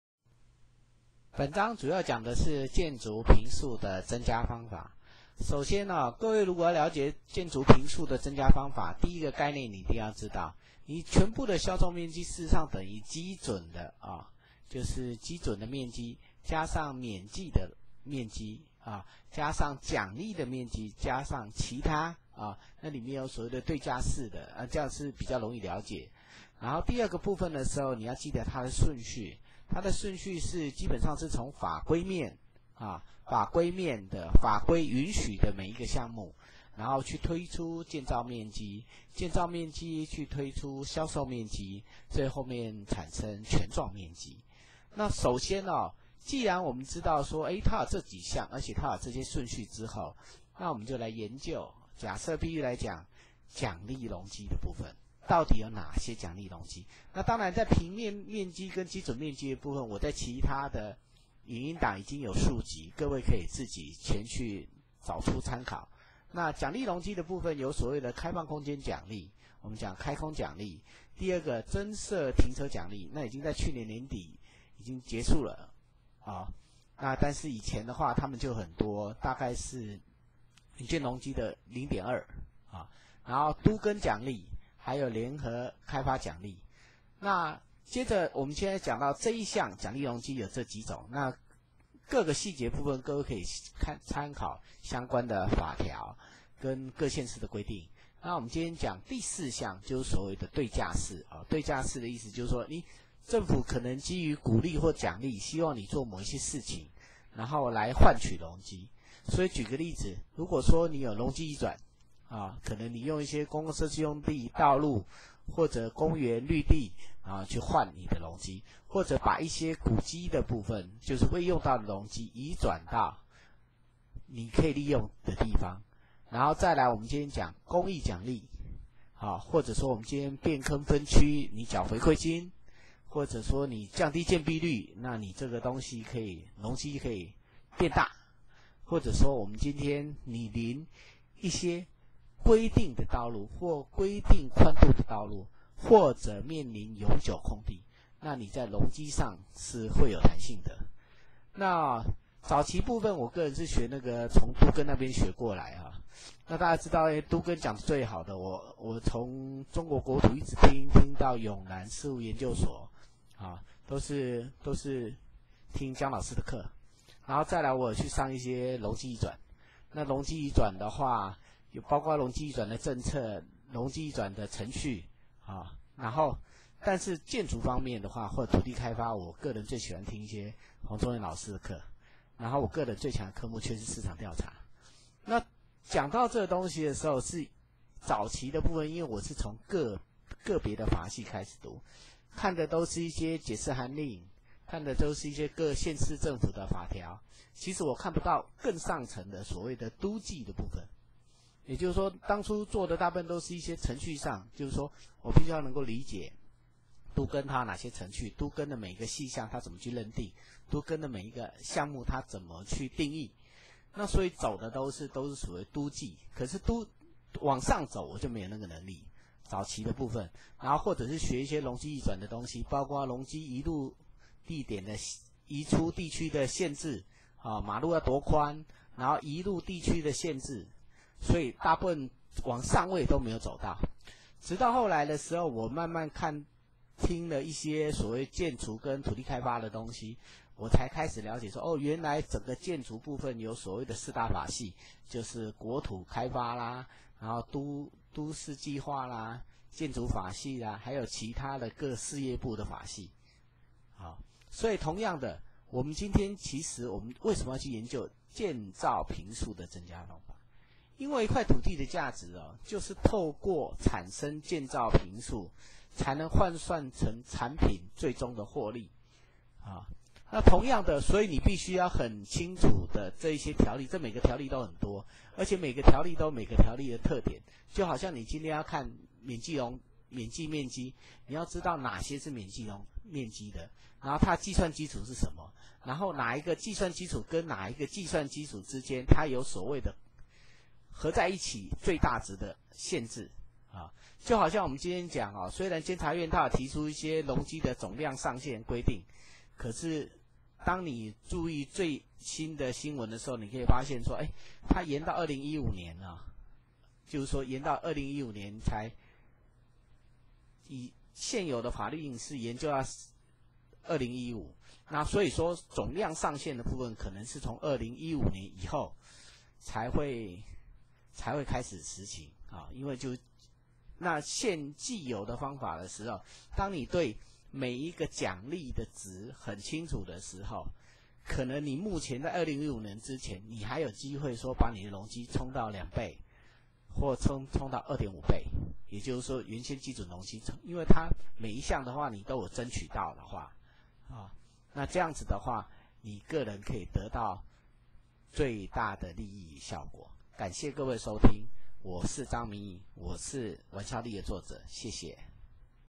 土地開發的建築坪數增加 (英語思維大突破,創業錢圖,創業計畫書,向塔木德學做租屋達人)
建築予建基準容積加免計與獎勵與其他,可以算出銷售面積!